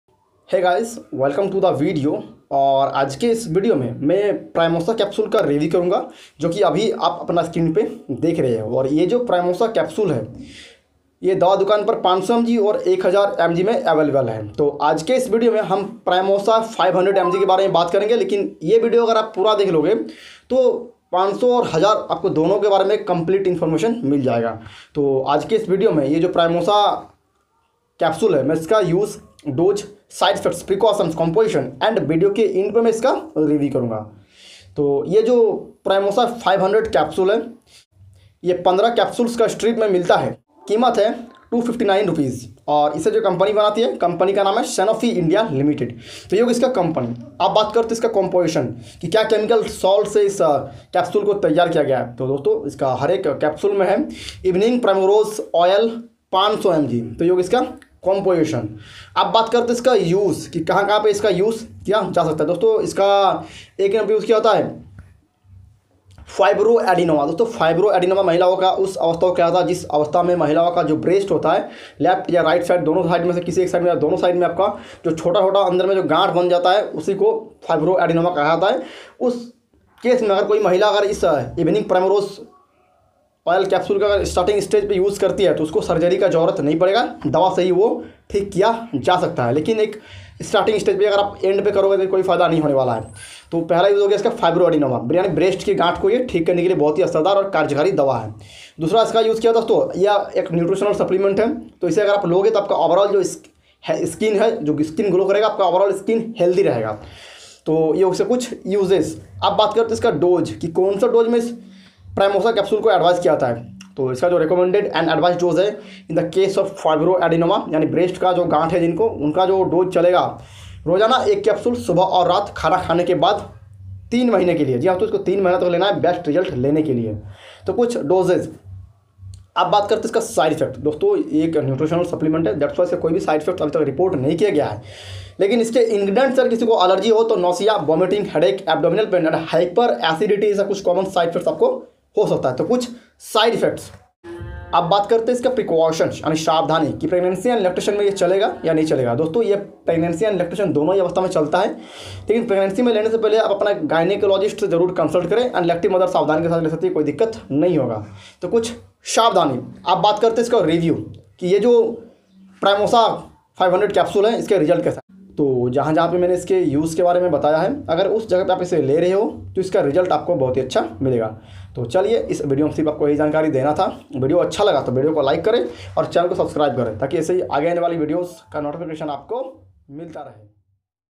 Primosa Capsule Use Dose Side-Effects Precautions And Review
Hello Dosto Aaj Ke Iss Video Me Mene Primosa Capsule Ka Use Dose Side-Effects Precautions And Review Ke Bare Me Bataya Hai Agar Aap Iss Jankari Ko Janana Chahte Ho To Video Ko Pura Dekhe.